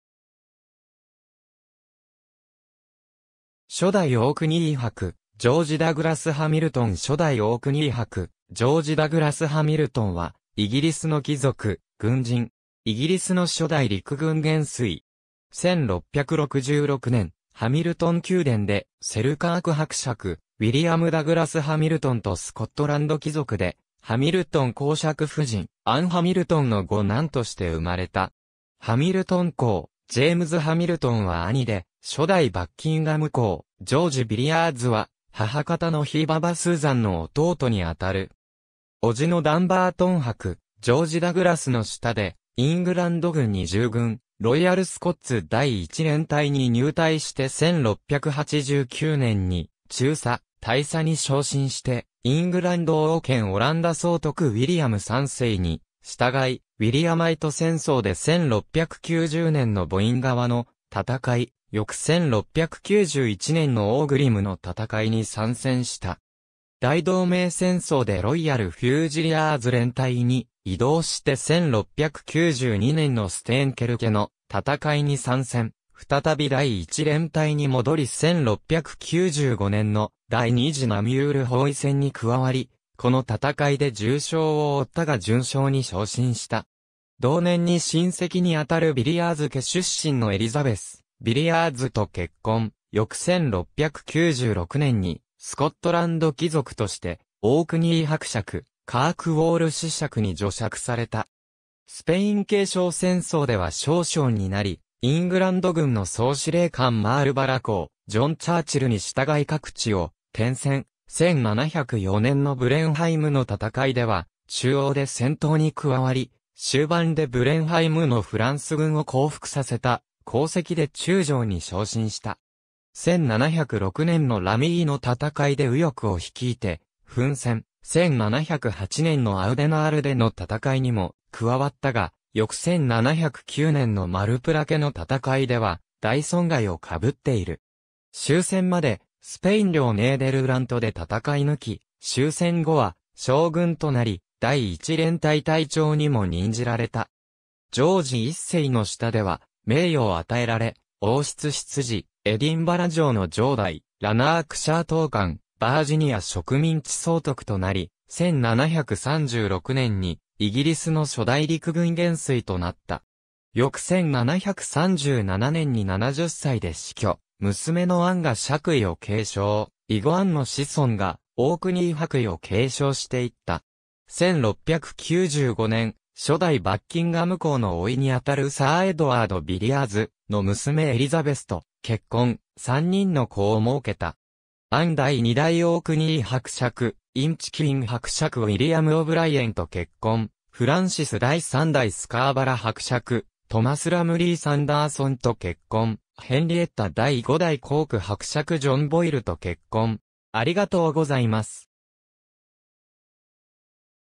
ジョージ・ダグラス＝ハミルトン (初代オークニー伯)
初代オークニー伯ジョージ・ダグラス＝ハミルトン 初代オークニー伯ジョージ・ダグラス＝ハミルトン（George Douglas-Hamilton, 1st Earl of Orkney, KT, 1666年2月9日 - 1737年1月29日）は、イギリスの貴族、軍人。
イギリスの初代陸軍元帥。
1666年、ハミルトン宮殿でセルカーク伯爵ウィリアム・ダグラス＝ハミルトンとスコットランド貴族でハミルトン公爵夫人アン・ハミルトンの5男として生まれた（母アンは自らの権利としてハミルトン公爵の称号を保持していた）。
ハミルトン公ジェイムズ・ハミルトンは兄で、初代バッキンガム公ジョージ・ヴィリアーズは母方の曾祖母スーザンの弟に当たる。
叔父のダンバートン伯ジョージ・ダグラスの下でイングランド軍に従軍、ロイヤル・スコッツ第1連隊に入隊して1689年に中佐、大佐に昇進してイングランド王兼オランダ総督ウィリアム3世に従いウィリアマイト戦争で1690年のボイン川の戦い、翌1691年のオーグリムの戦いに参戦した。
大同盟戦争でロイヤル・フュージリアーズ連隊に異動して1692年のステーンケルケの戦いに参戦、再び第1連隊に戻り1695年の第二次ナミュール包囲戦に加わり、この戦いで重傷を負ったが准将に昇進した。
同年に親戚に当たるヴィリアーズ家出身のエリザベス・ヴィリアーズと結婚、翌1696年にスコットランド貴族としてオークニー伯爵、カークウォール子爵に叙爵された。
スペイン継承戦争では少将になりイングランド軍の総司令官マールバラ公ジョン・チャーチルに従い各地を転戦、1704年のブレンハイムの戦いでは中央で戦闘に加わり、終盤でブレンハイムのフランス軍を降伏させた功績で中将に昇進した。
1706年のラミイの戦いで右翼を率いて奮戦、1708年のアウデナールデの戦いにも加わったが、翌1709年のマルプラケの戦いでは大損害を被っている。
終戦までスペイン領ネーデルラントで戦い抜き、終戦後は将軍となり第1連隊隊長にも任じられた。
ジョージ1世の下では名誉を与えられ王室執事、エディンバラ城の城代、ラナークシャー統監、バージニア植民地総督となり、1736年にイギリスの初代陸軍元帥となった。
翌1737年に70歳で死去、娘のアンが爵位を継承、以後アンの子孫がオークニー伯位を継承していった。
1695年、初代バッキンガム公の甥に当たるサー・エドワード・ヴィリアーズの娘エリザベスと結婚、3人の子を儲けた。
アン（? - 1756年） - 第2代オークニー伯爵、インチクィン伯爵ウィリアム・オブライエンと結婚 フランシス（? - 1772年） - 第3代スカーバラ伯爵トマス・ラムリー＝サンダーソンと結婚 ヘンリエッタ（? - ?） - 第5代コーク伯爵ジョン・ボイルと結婚